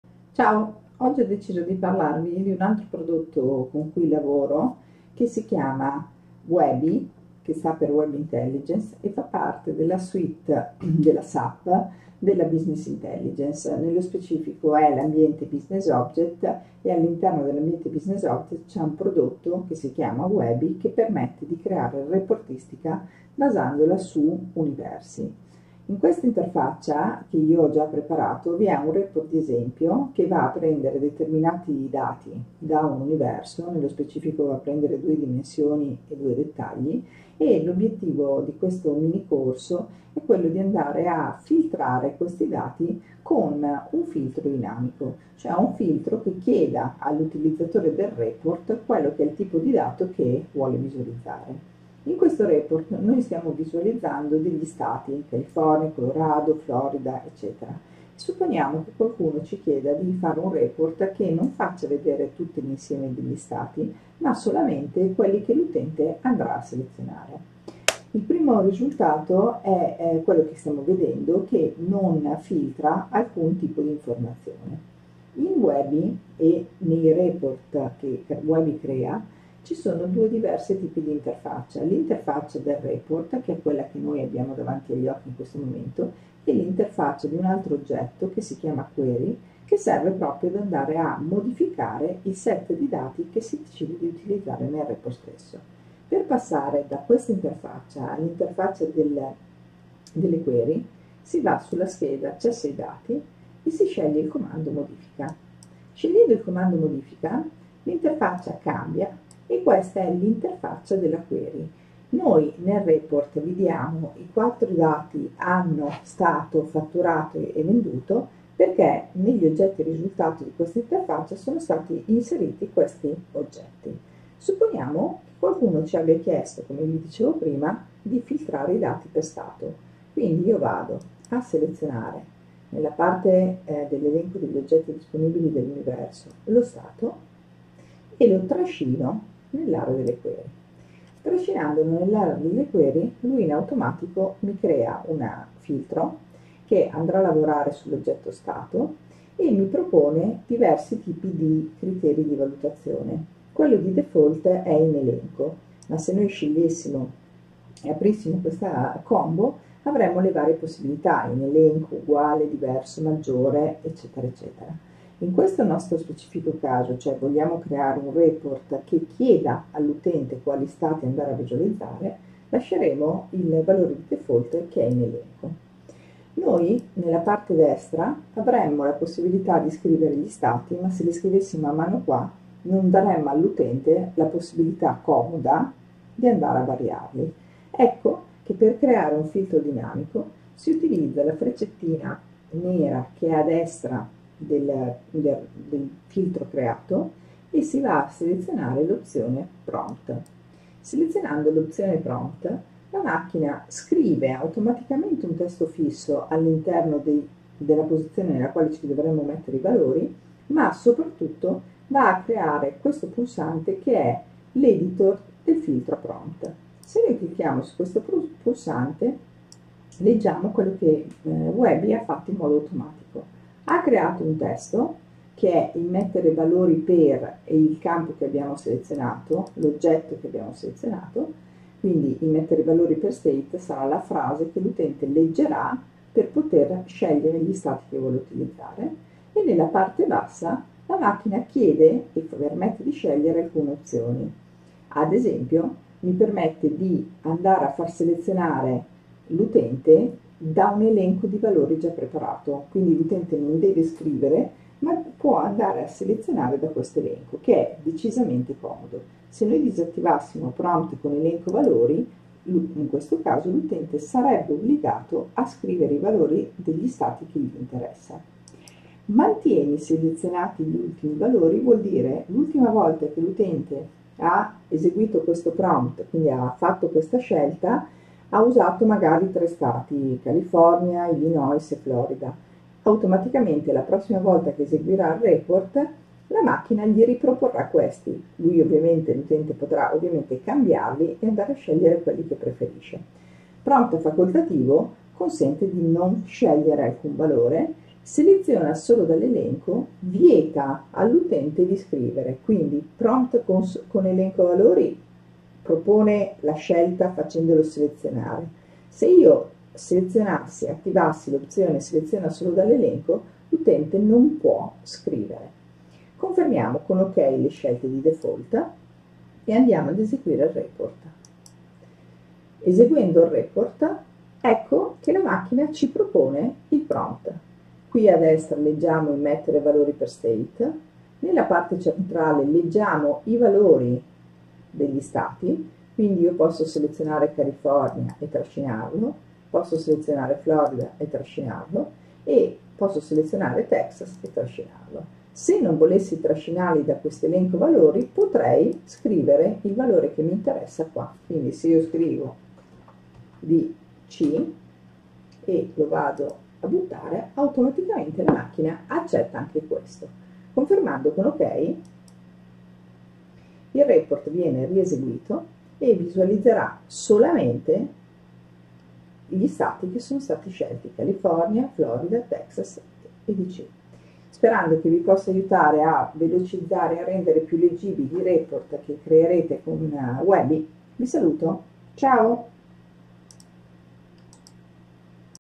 Impostare un filtro dinamico all'interno di un report creato con WEBI
Hai creato un report con WEBI e vuoi renderlo dinamico? Vuoi che in fase di apertura il report ti ponga una domanda ed utilizzi la risposta come filtro? Guarda il prossimo video!!